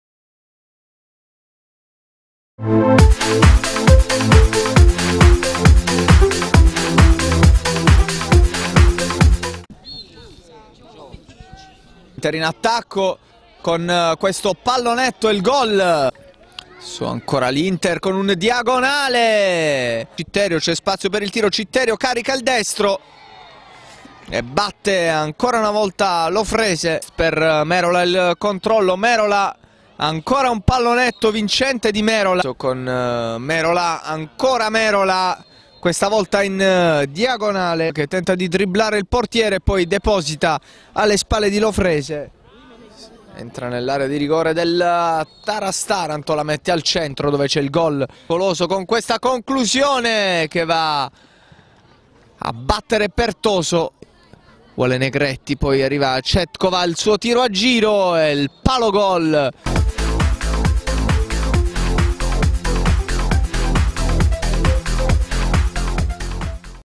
X Trofeo Carnevale di Gallipoli - Inter vs.Taras Taranto 9-0
Prova di forza dell'Inter che ipoteca la qualificazione nel girone B della X edizione del Trofeo Carnevale di Gallipoli-Coppa Santa Maria di Leuca. I nerazzurri di mister Mandelli battono 9-0 la Nuova Taras Taranto in un match chiuso praticamente dopo un quarto d'ora. Mvp del match Merola, autore di due pregevoli cucchiai che si sono insaccati alle spalle del portiere avversario. Proprio Merola ha sbloccato il punteggio dopo 6' controllando la sfera e, dopo aver dato un'occhiata al piazzamento di Lofrese, scavalcandolo con un pallonetto. Dopo cinque minuti il raddoppio di Mendoza, bravo a recuperare palla sulla tre quarti e, dopo essersi portato il pallone avanti di qualche metro a scagliare un potente diagonale che batte il portiere pugliese. Azione da manuale per il gol di Citterio, che dal centro dell'area infila di giustezza. Ancora Merola mette a segno il quarto e il quinto gol prima che Goven timbri il cartellino a pochi minuti dal termine del primo tempo. Nella ripresa spazio per le segnature di Negretti, Patacchini e Tchetchova, che chiudono il match sul 9-0.